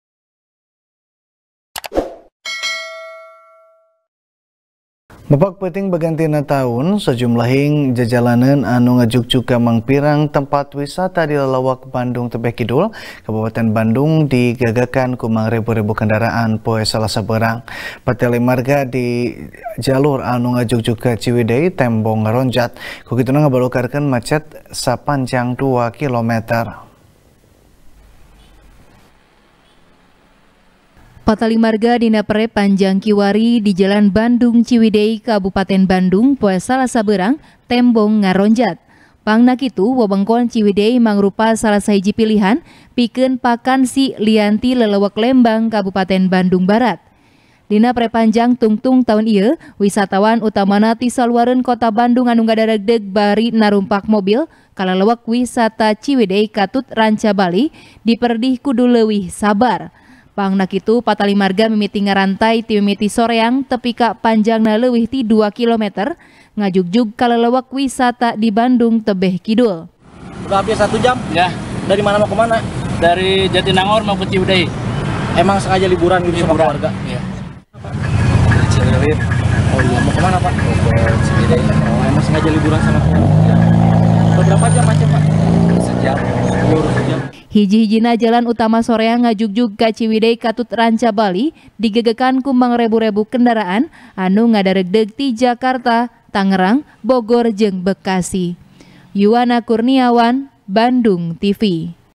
Mapag Peuting Bagantina Taun Jalur Soreang - Ciwidey Macet | TP | BANDUNGTV
TP | BANDUNGTV | DERI FIRMAN | Mapag Peuting Bagantina Taun, Sajumlahing Jajalaneun Anu Ngajugjug Ka Mangpirang Tempat Wisata Di Lelewek Bandung Tebeh Kidul, Kabupaten Bandung, Digegekan Ku Mangrebu-Rebu Kendaraan, Poe Salasa Beurang. Patalimarga Dina Jalur Anu Ngajugjug Ka Ciwidey, Tembong Ngaronjat. Ku Kituna, Ngabalukarkeun Macet Sapanjang Dua Kilometer.